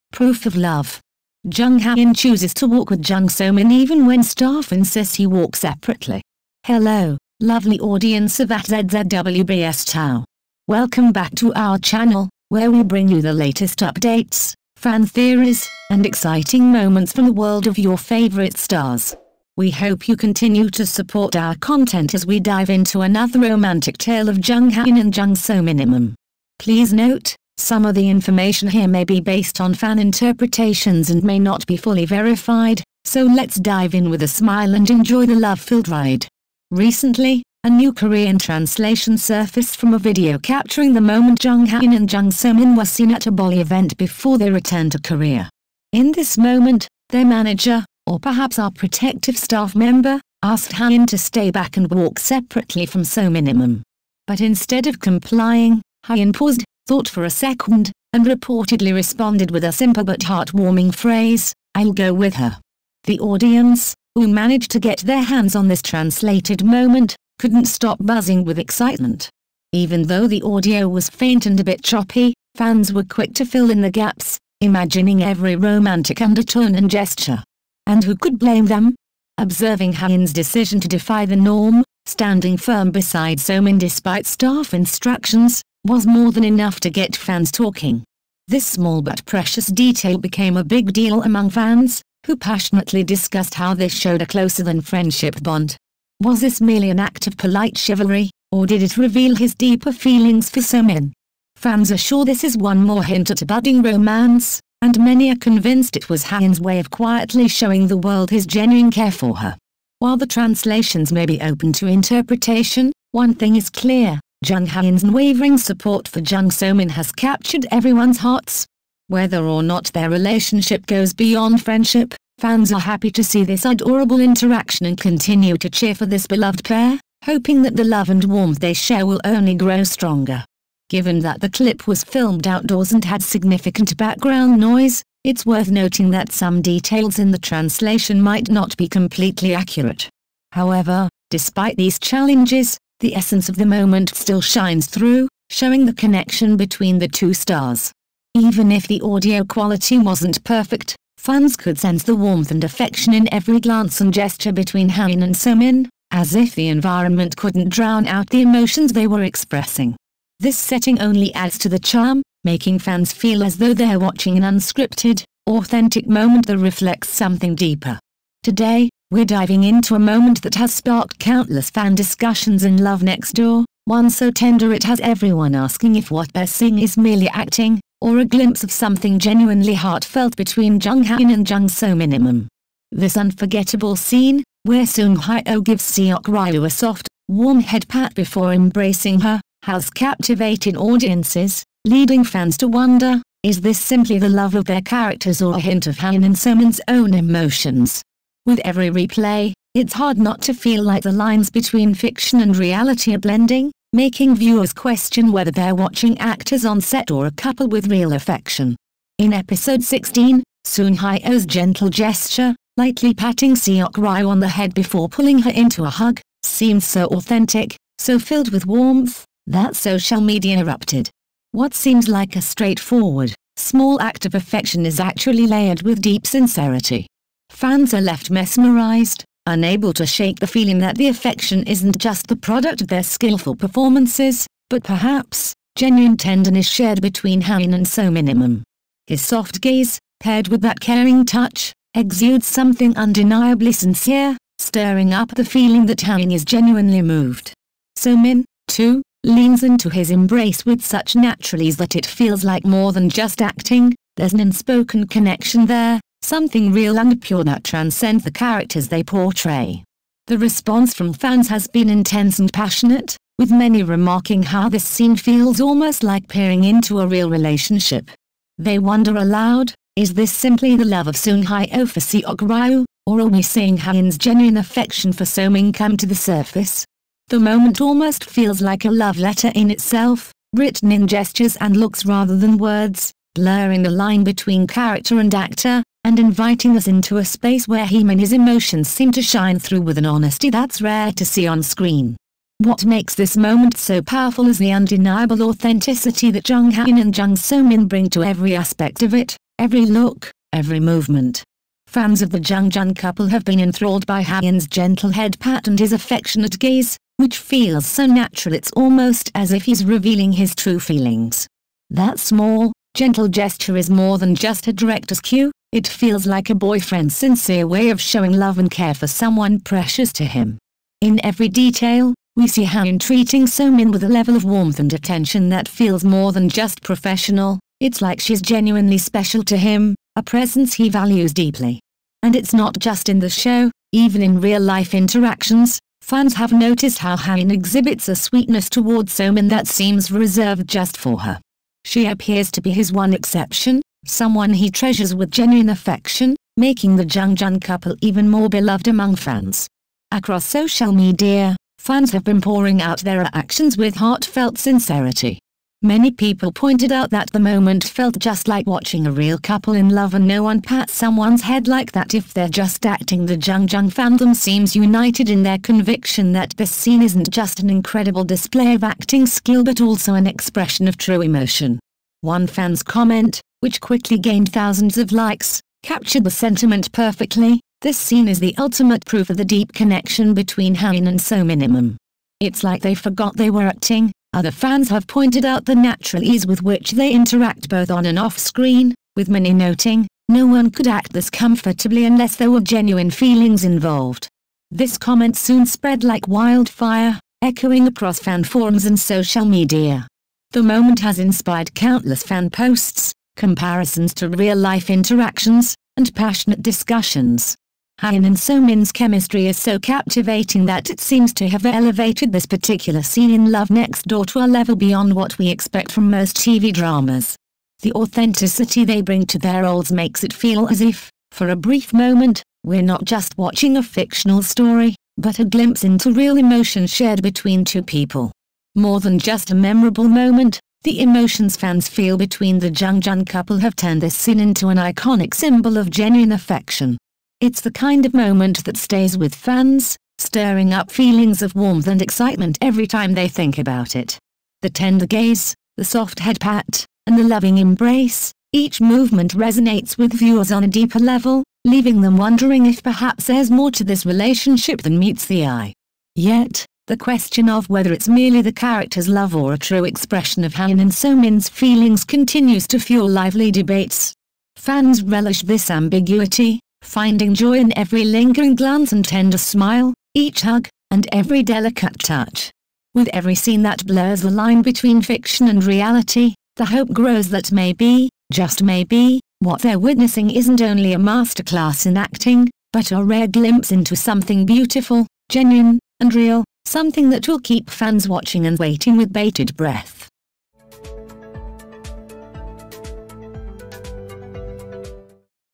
Proof of Love! Jung Hae In Chooses to Walk Beside Jung So Min Despite Staff Requests!
Hello, lovely audience of @ZZW_BSThao! Please note, some of the information here may be based on fan interpretations and may not be fully verified, so let’s dive in with a smile and enjoy the love-filled ride!